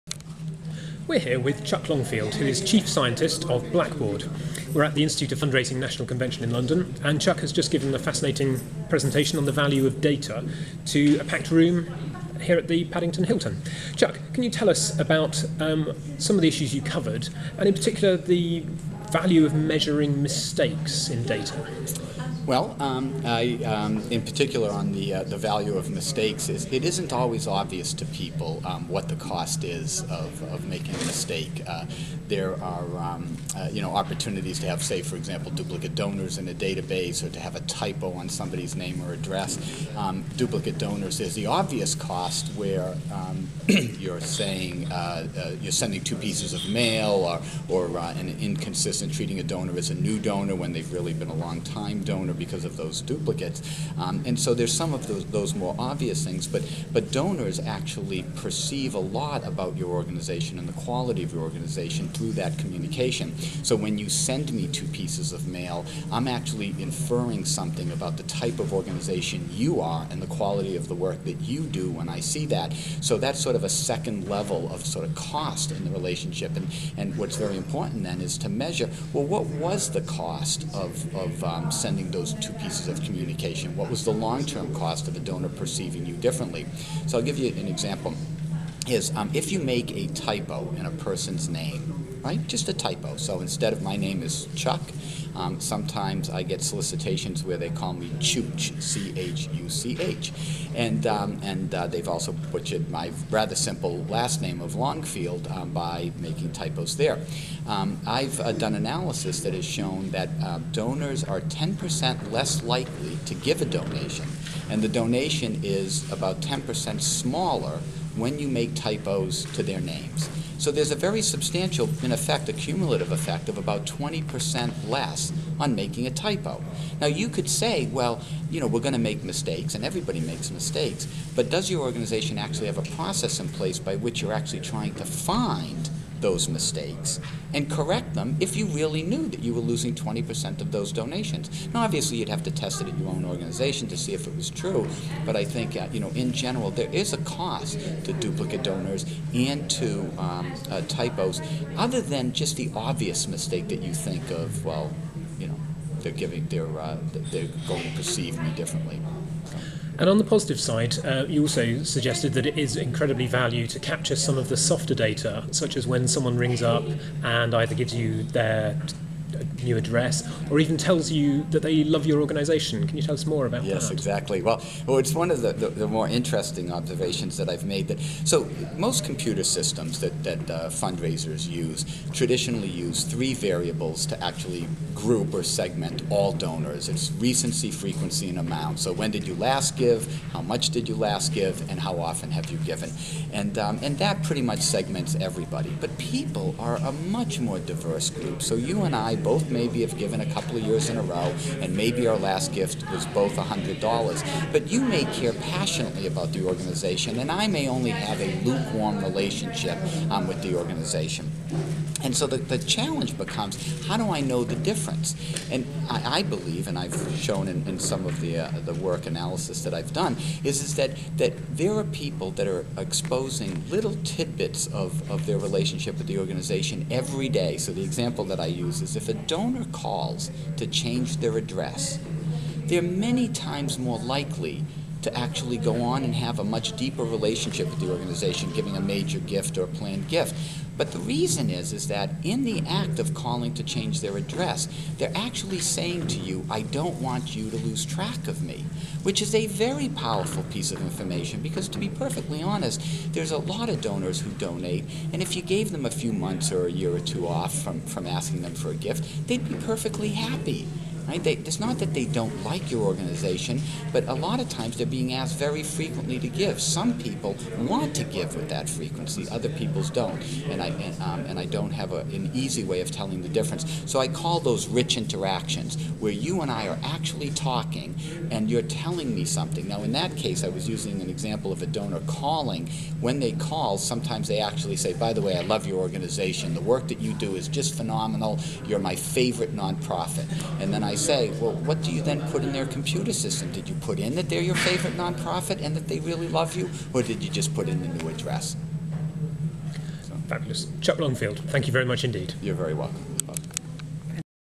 Interview with Chuck Longfield, Blackbaud Chief Scientist | UK Fundraising
Interview with Chuck Longfield, Chief Scientist at Blackbaud Inc, at the Institute of Fundraising National Convention, 5 July 2010, London. Interview by Howard Lake, UK Fundraising

He points out that regular donors are quite diverse. Some are genuinely committed to your cause, others much less.

Most give hints though. If someone calls up to let you know that they are changing their address, they are truly valuable. But how do charities note down this interaction in their supporter database?